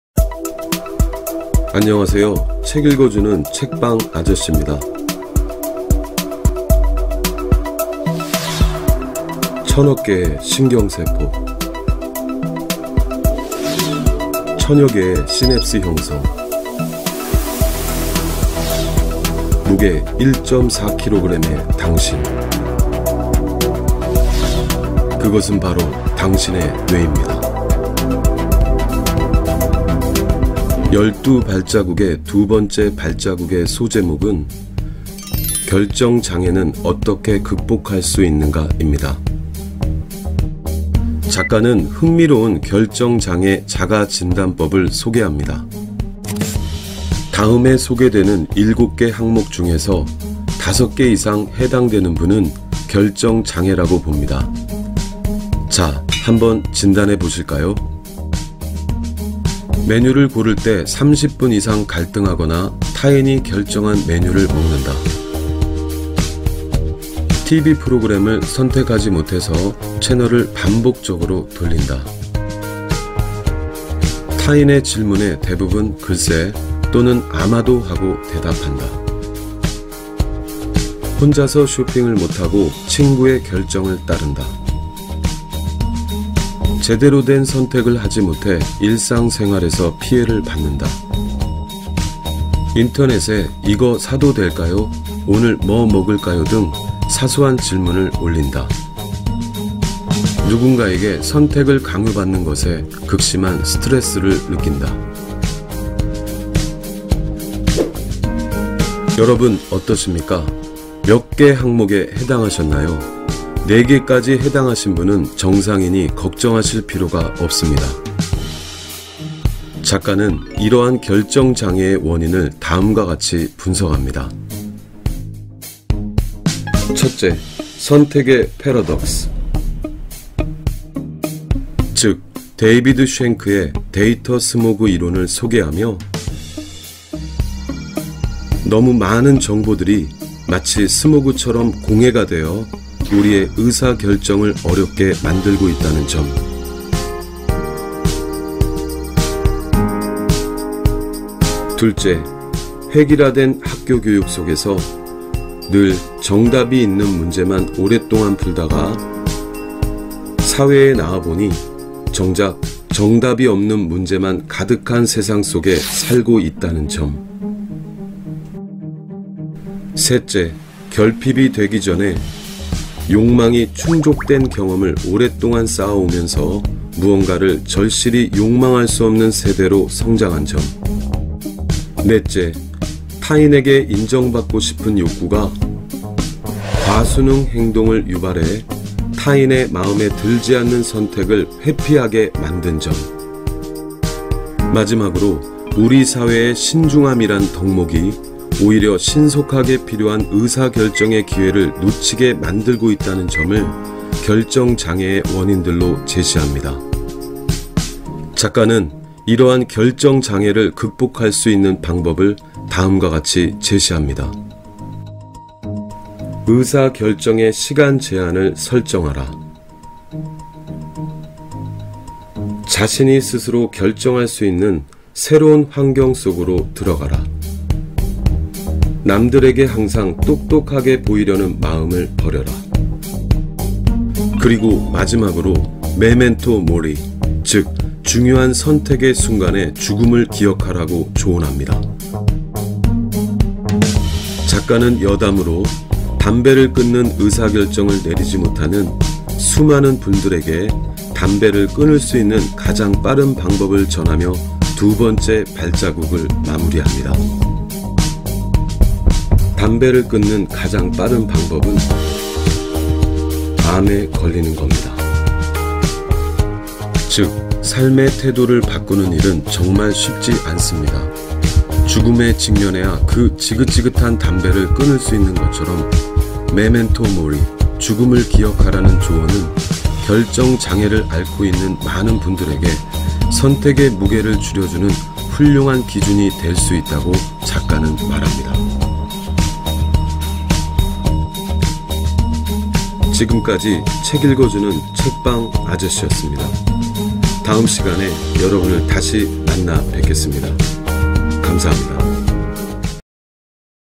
열두 발자국 | 결정장애는 어떻게 극복할 수 있는가? | 정재승 | 책방아저씨
결정장애 | 열두 발자국 | 정재승

삶의 태도를 바꾸는 일은 정말 쉽지 않습니다.

죽음에 직면해야 그 지긋지긋한 담배를 끊을 수 있는 것처럼, ‘메멘토 모리’, 죽음을 기억하라! 는 조언은 결정장애를 앓고 있는 많은 분들에게 선택의 무게를 줄여주는 훌륭한 기준이 될 수 있다고 작가는 말합니다.

Kaist 대학교 정재승 교수님의 "열두 발자국" 중 [두 번째 발자국] : 결정장애는 어떻게 극복할 수 있는가?

 '책방 아저씨'가 그 대책을 읽어 드립니다.